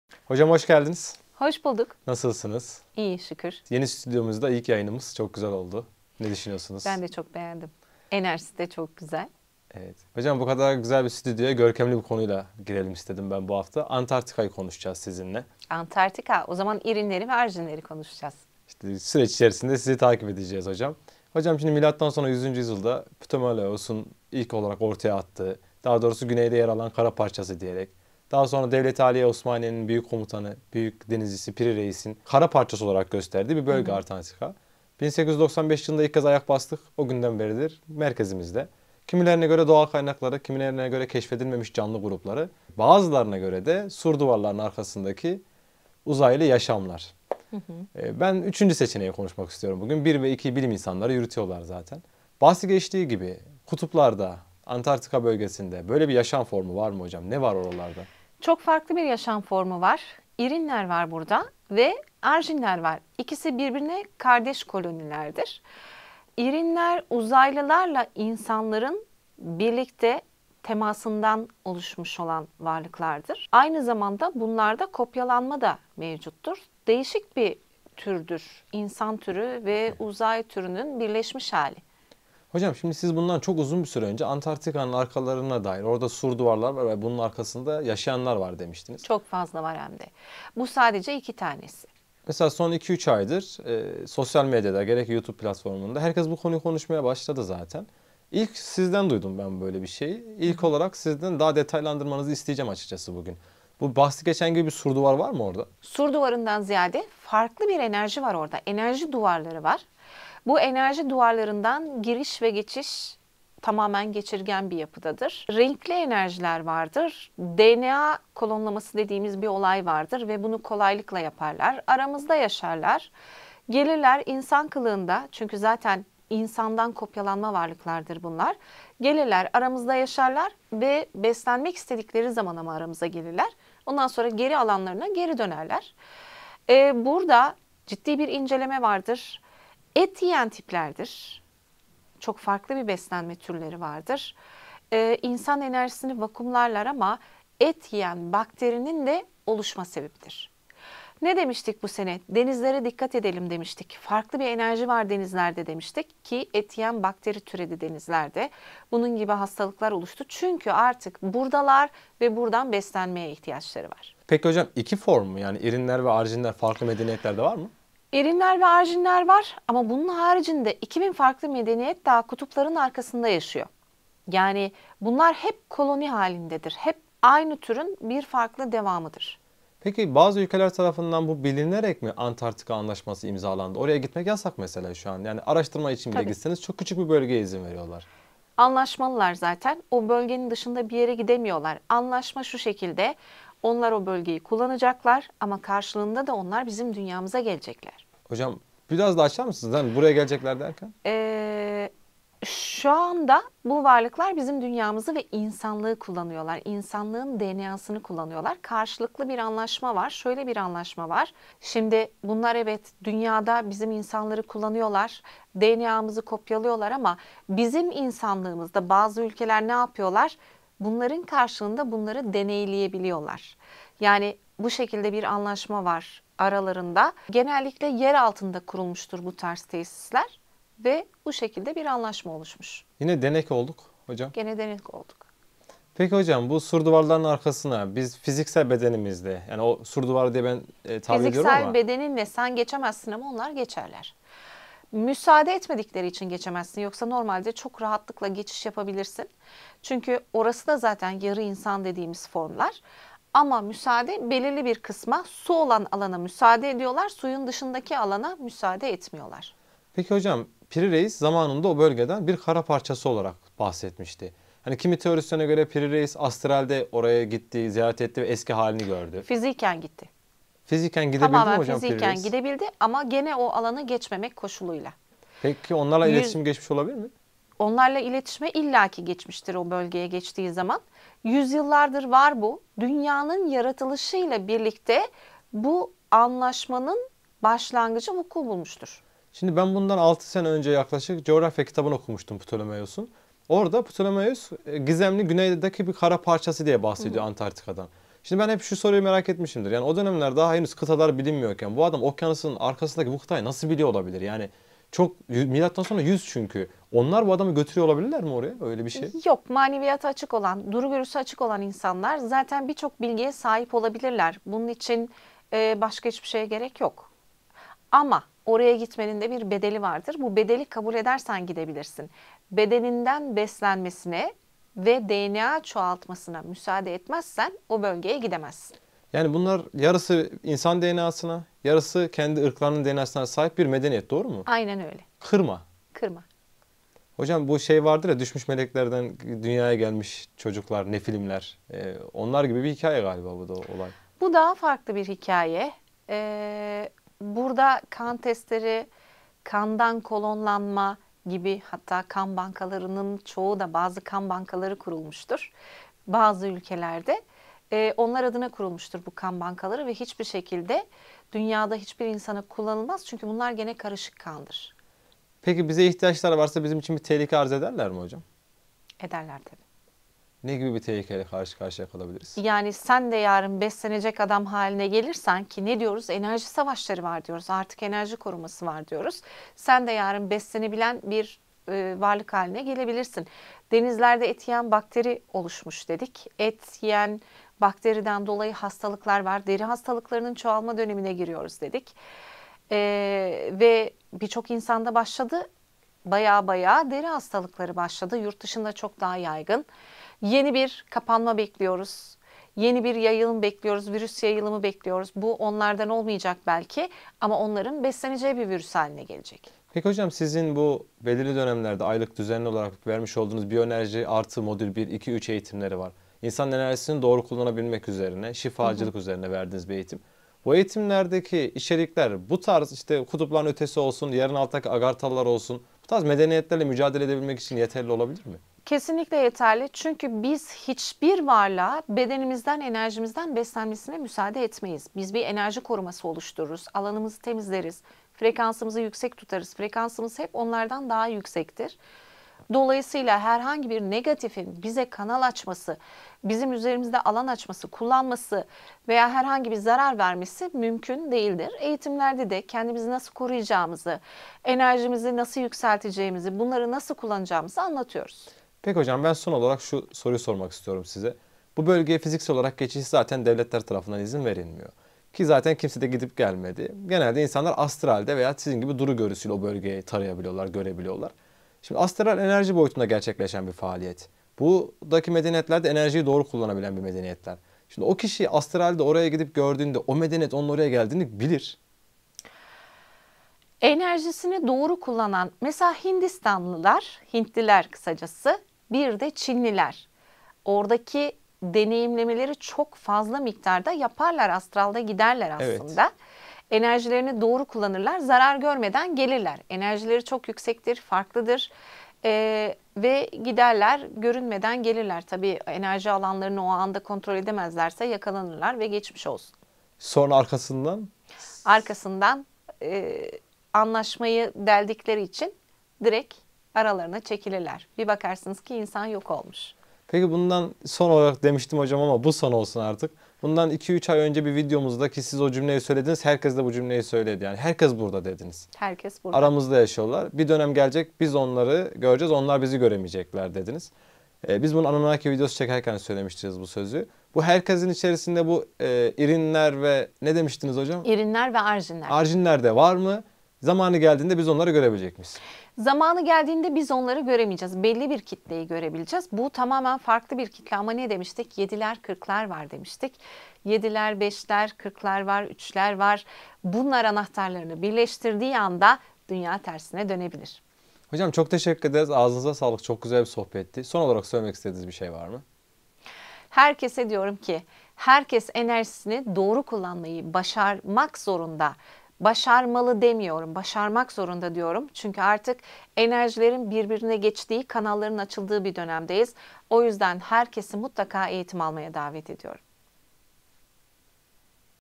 Maria ÖCAL - İriynler & Arjiynler Kim?
Yıllardır gizemin koruyan Antartika !
Varlığı iddia edilen sur duvarları !
Arkasında ne var ?
Büyük devletler neden Antartika'ya giriş çıkışa izin vermiyor ?
Tüm bu sorularınızın cevabını öğreneçeğiniz, daha önce hiç bir yerde duymadıklarınızı duyacağınız program başlıyor. Dr. Maria Öcal Antartika'da ki gizem perdesini aralıyor.
İRİYNLER ve ARJİYNLER kimdir ?